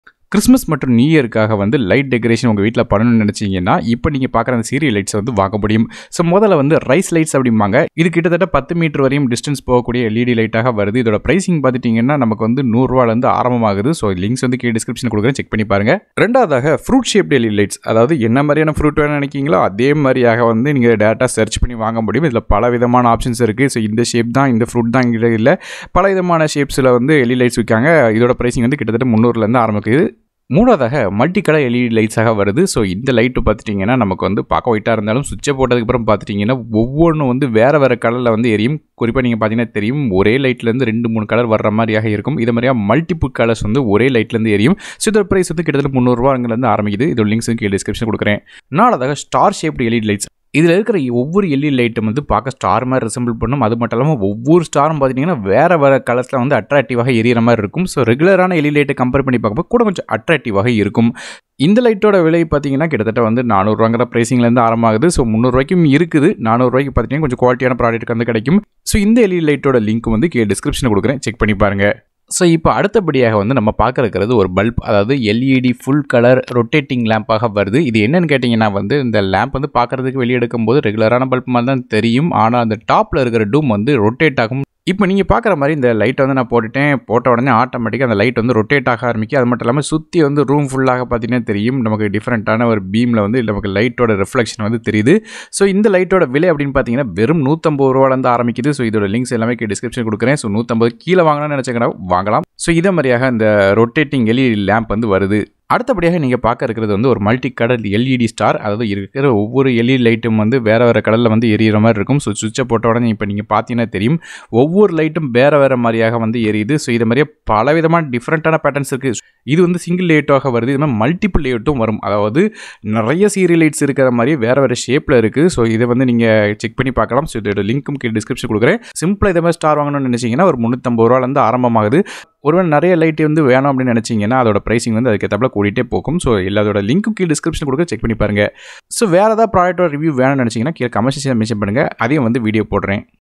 LED Serial Lights for Christmas 🎄 LED Rotating Bulb & Star Review 🎅
Rice Lights for New Year Party  stage decoration etc

Rice Serial Lights
Multi Colored Lights
Fruit Shaped Lights
Star Shaped
Rotating LED Bulb
LED Stars